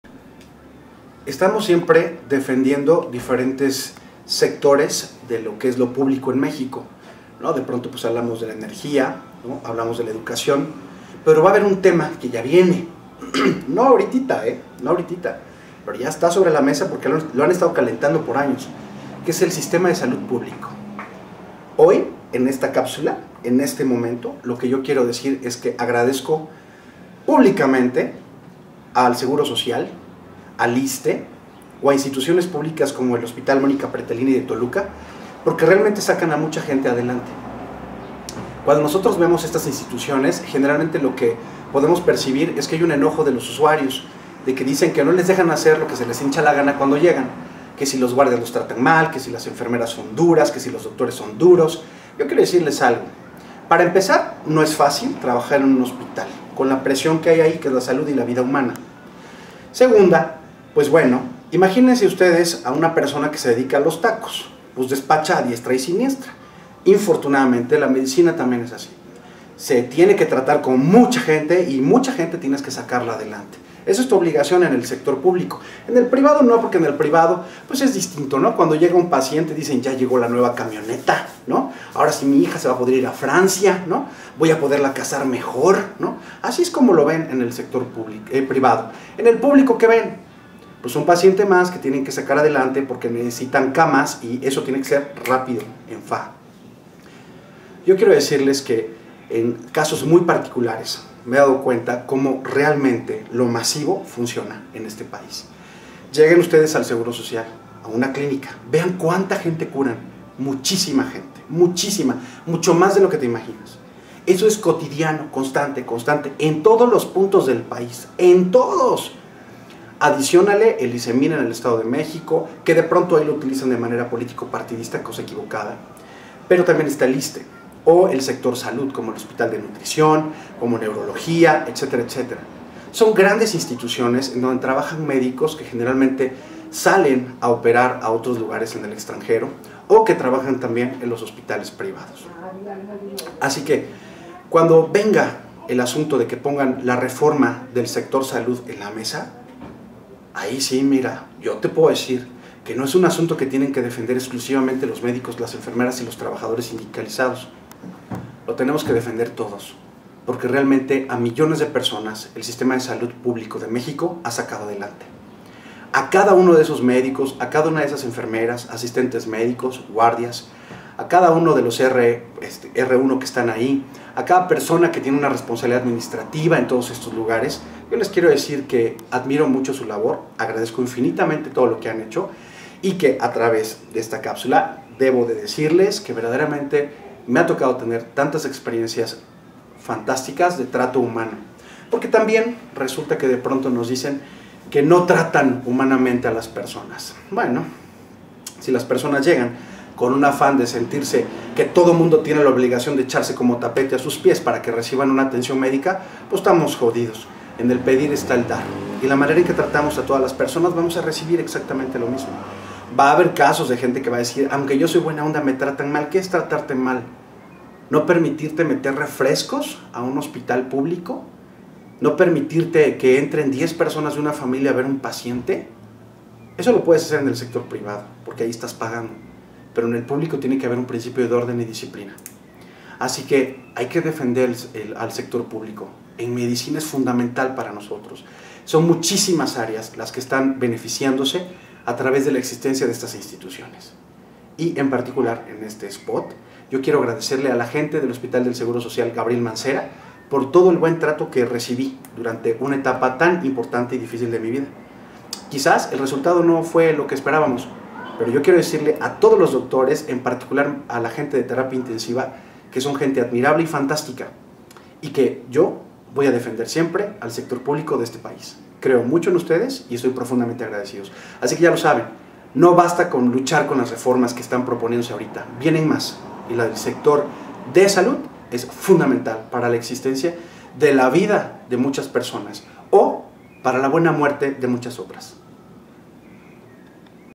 En Defensa del IMSS
Muchos despotrican contra el IMSS, ISSSTE y otras instituciones públicas. Esto es precisamente lo que busca el gobierno federal, pues los ciudadanos ROGARAN que sean PRIVATIZADOS. ¿Te has puesto a pensar qué hay atrás de esto? Hay personas que SI APOYAN y ponen SU CORAZON para sanar personas. No te vayas con la finta. EPN y demás LACRAS solo buscan deteriorar lo tuyo para VENDERLO y encarecer lo que por derercho te toca.